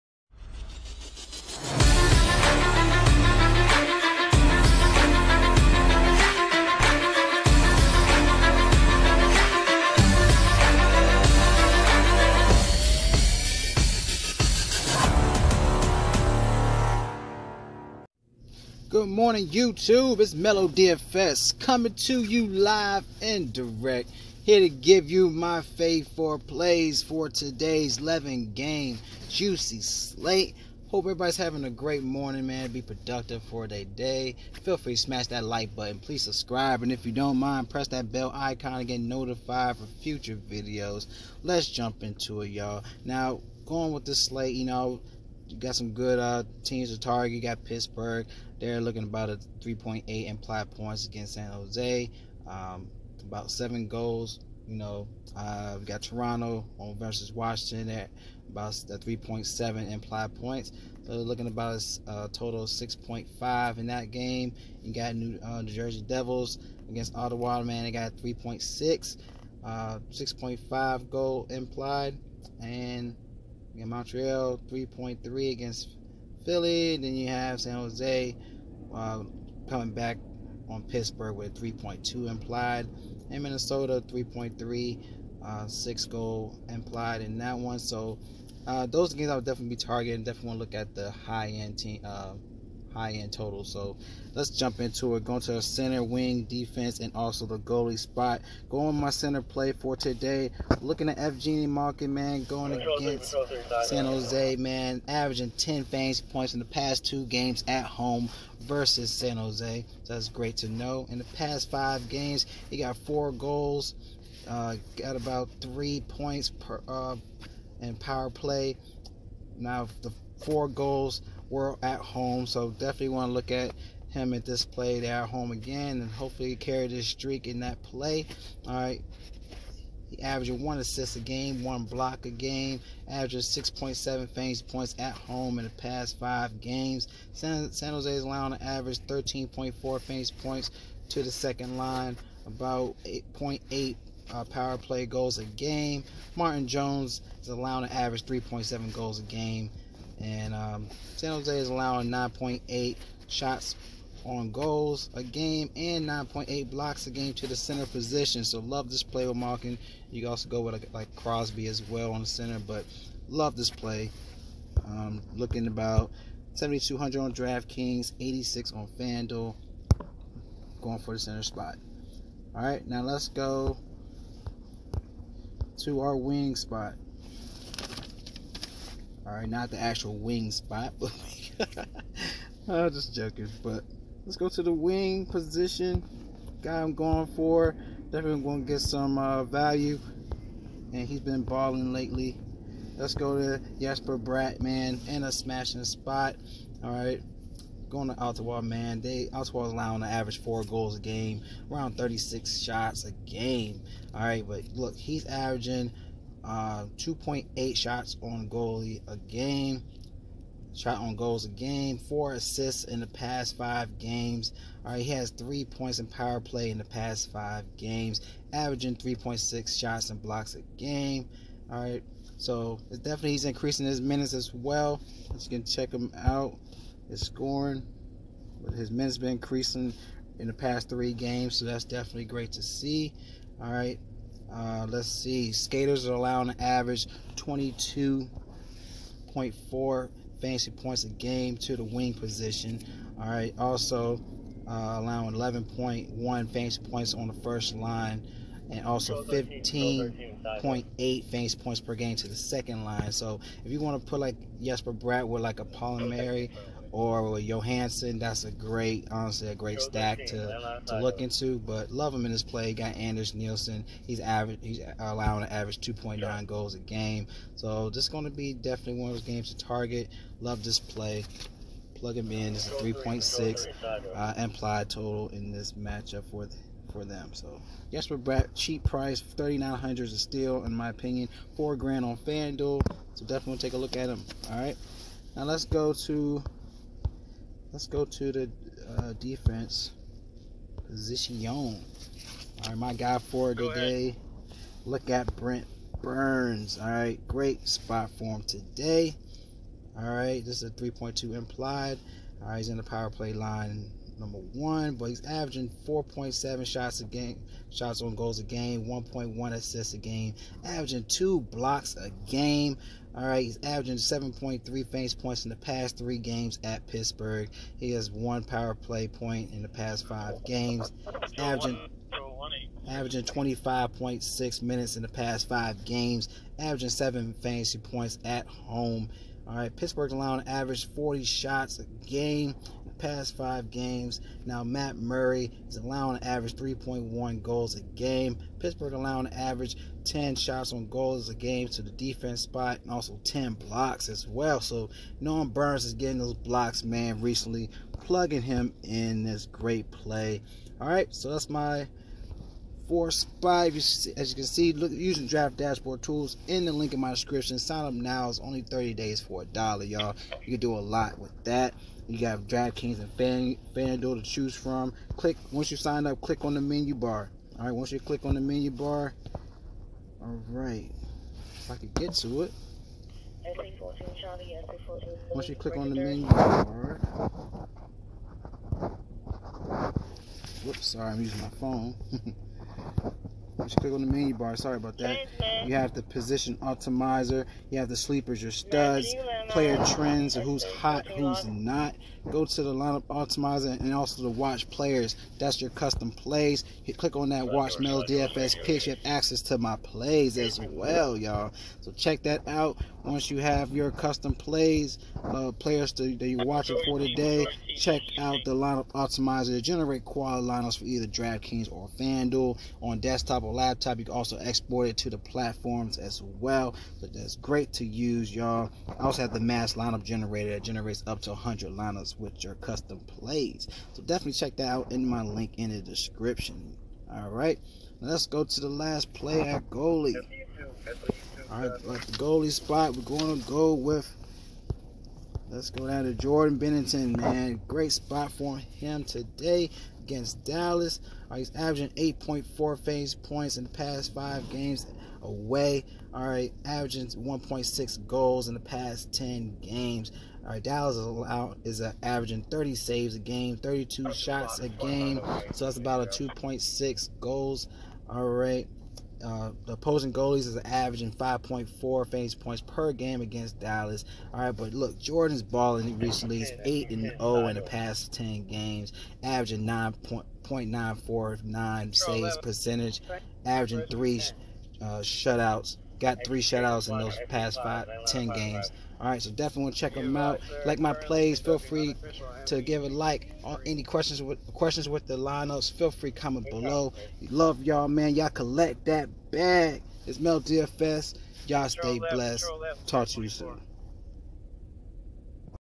NHL DFS DraftKings Picks + FanDuel Picks Tonight- 2/21/2019
M3lo's Favorite 4 picks for NHL MAIN SLATE

RECEIVE MY DAILY PLAYER POOL

Will provide updated FAV 5 before lockdown on the same video and Twitter, so ADD me on Twitter @m3lorama